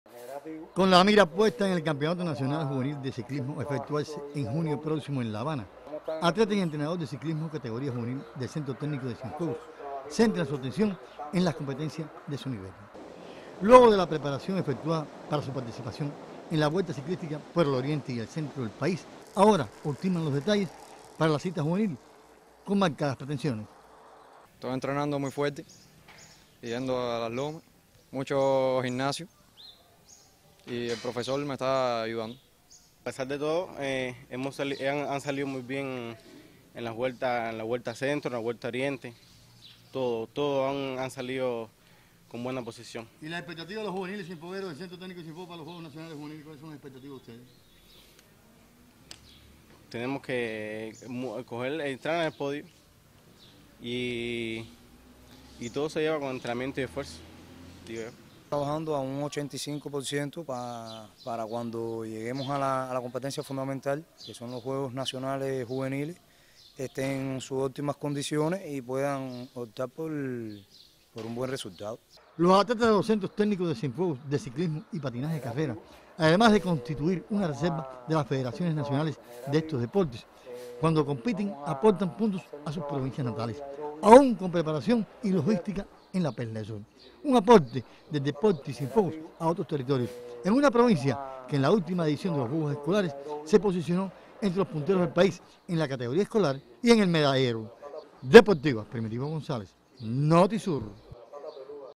En preparación ciclistas juveniles en Cienfuegos con vista a Torneo Nacional de la disciplina
Los atletas y entrenador de ciclismo juvenil del Centro Técnico de  Cienfuegos, se preparan con vista al Torneo Nacional de esa disciplina. El evento deportivo se efectuará en La Habana, el próximo mes de junio.